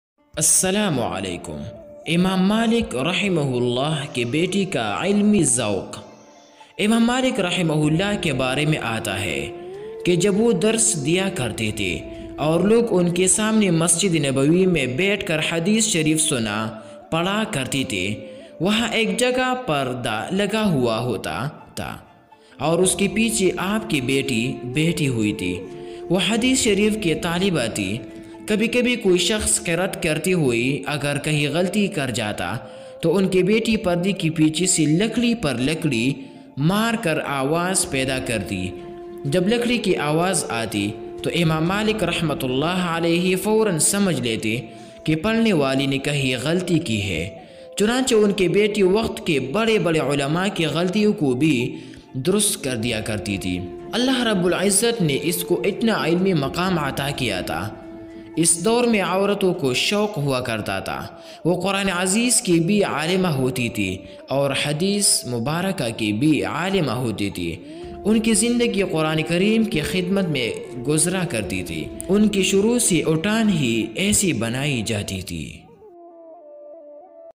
Imam Malik (RA) ki Baity | Latest Video | Very Heart Touching Video | Hindi World | اردو کہانی
Video Voice Speaker : Muhammad Sajjad
Video Editor : Muhammad Sajjad
Channel Name : Hindi World
Country : Pakistan

I Love Pakistan And All Muslims .

 امام مالك رح کی بیٹی کا علمی ذوق __!!
امام مالك رحمہ اللہ کے بارے میں آتا ہے کہ جب وہ درس دیا کرتے تھے اور لوگ ان کے سامنے مسجد نبوی میں بیٹھ کر حدیث شریف سُنا، پڑھا کرتے تھے۔وہاں ایک جگہ پردہ لگا ہوا ہوتا تھا اور اس کے پیچھے آپ کی بیٹی بیٹھی ہوتی تھی۔ وہ حدیث شریف کی طالبہ تھی۔کبھی کبھی کوئی شخص قرأت کرتے ہوئے اگر کہیں غلطی کر جاتا تو ان کی بیٹی پردے کے پیچھے سے لکڑی پر لکڑی مار کر آواز پیدا کرتی۔ جب لکڑی کی آواز آتی تو امام مالك رحمۃ اللہ علیہ فوراٗ سمجھ لیتے کہ پڑھنے والے نے کہیں غلطی کی ہے۔ چنانچہ ان کی بیٹی وقت کے بڑے بڑے علماء کی غلطیوں کو بھی درست کردیا کرتی تھی۔ اللہ رب العزت نے اسکو اتنا علمی مقام عطا کیا تھا!! اس  دور میں عورتوں کو شوق ہوا کرتا تھا۔ وہ قرآن عزیز کی بھی عالمہ ہوتی تھیں اور حدیث مبارکہ کی بھی عالمہ ہوتی تھیں۔ ان کی زندگی قرآن کریم کی خدمت میں گزرا کرتی تھی۔ ان کی شروع سے اٹھان ہی ایسی بنائی جاتی تھی۔۔۔!!
🎤 حضرت مولانا پیر ذوالفقار احمد صاحب نقشبندی دامت برکاتہم۔۔!!